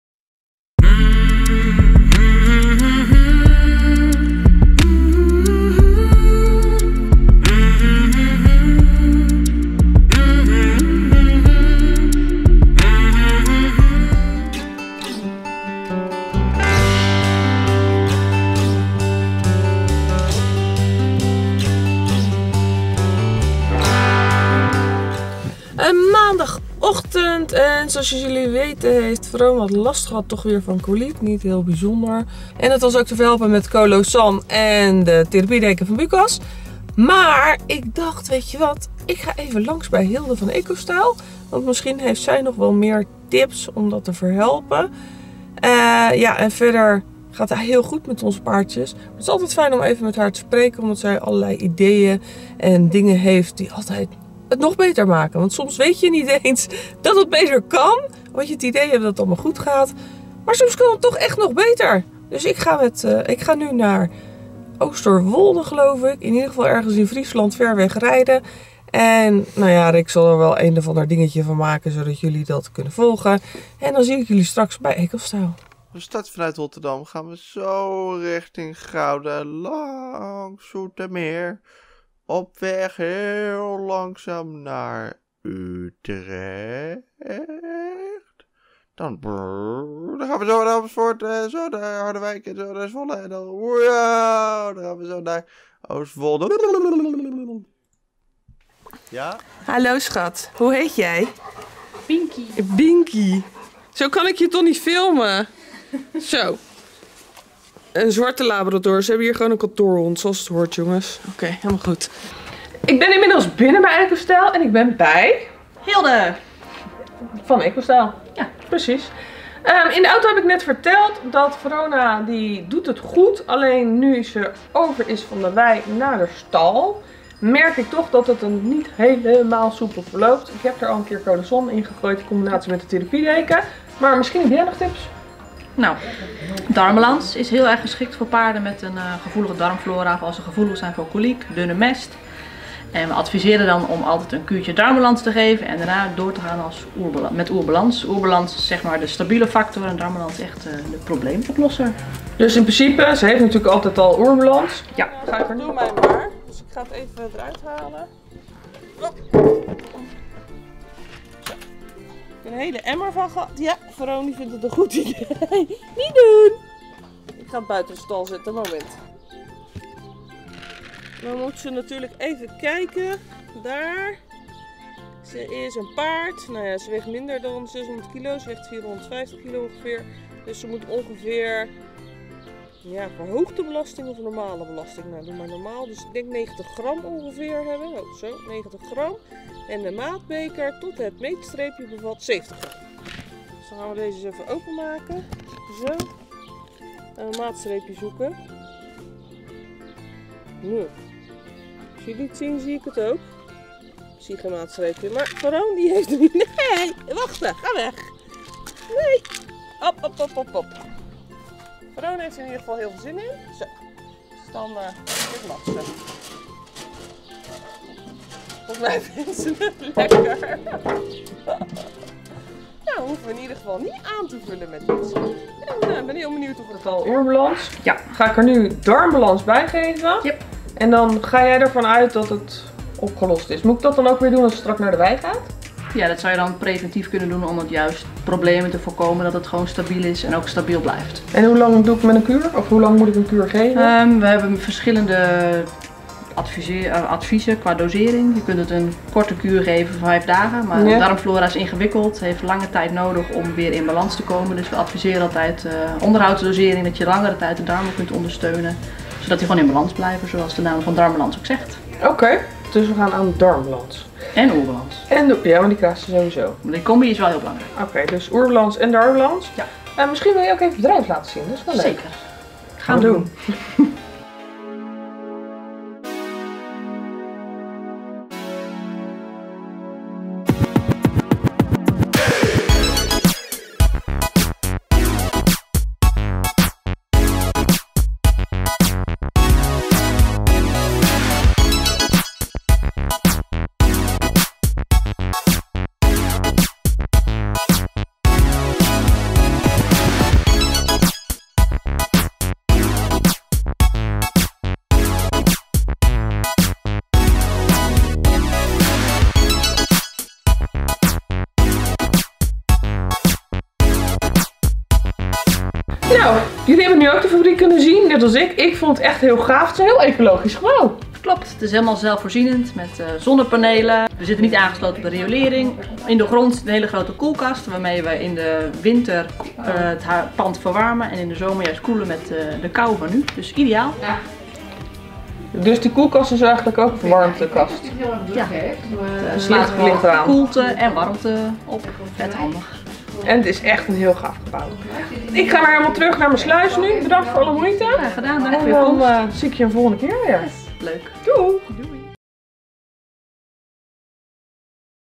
Op Bezoek bij ECOstyle * Hart voor Paarden*
Ik was nog niet bij ECOstyle langs geweest, dus het was hoog tijd voor een bezoek aan ECOstyle. Hilde heeft me rondgeleid door het bedrijf en kwam nog met een goede tip voor Verona!

Ook post sturen? Dat kan naar:
Honderdland 9d
2676LT Maasdijk

Kijk de nieuwe weekvlog!: "Weekvlog 7 december 2019"